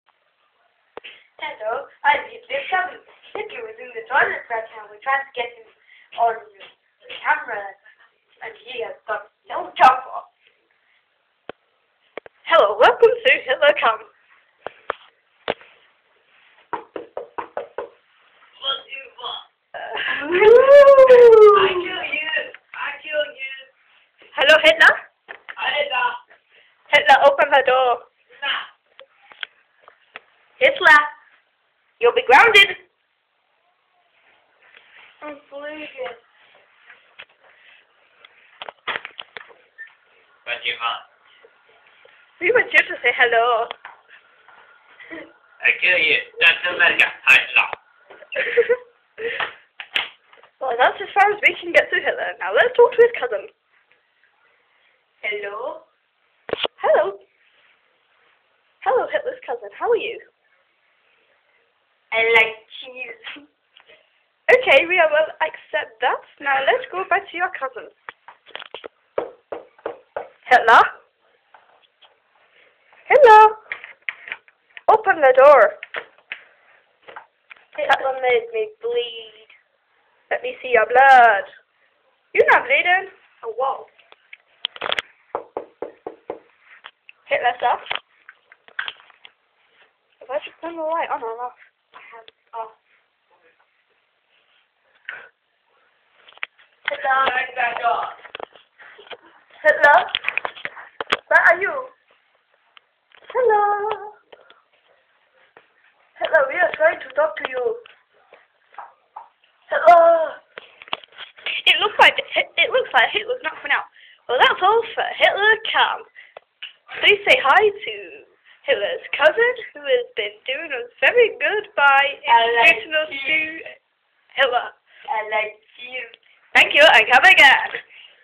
Hitler Cam : D
Hitler cam is here for the 1st time, with me as narator & Beth as Hitler's cousin.
Bradley Tetlow as HITLER